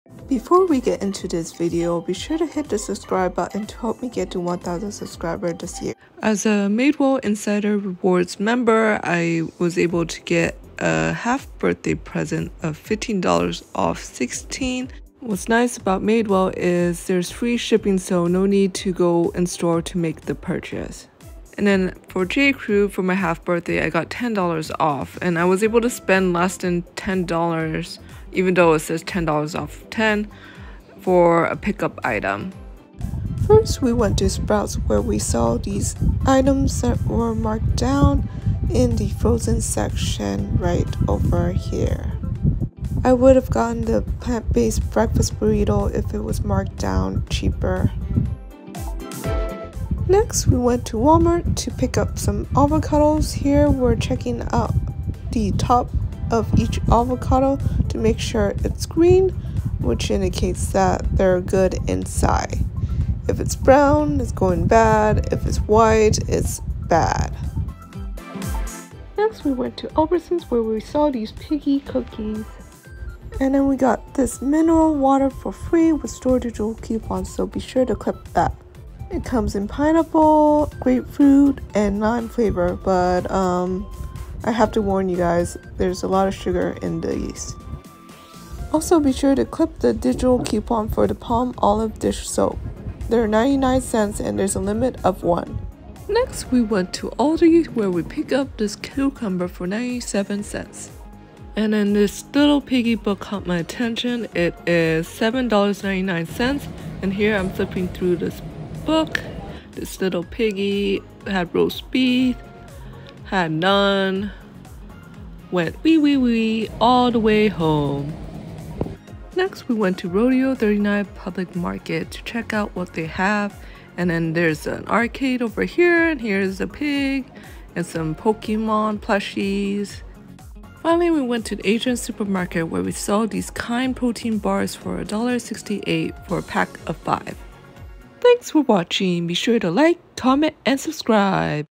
Sept 14 | Grocery and Non-Grocery Freebies and Deals! | Half Birthday and Non Birthday Deals
In this video, we went to multiple stores and got a bunch of freebies and deals. I redeemed a couple half birthday deals/freebies I got. I’ll post more videos when we get more freebies/deals.

To learn how I save money here and there, check out my videos and my blog her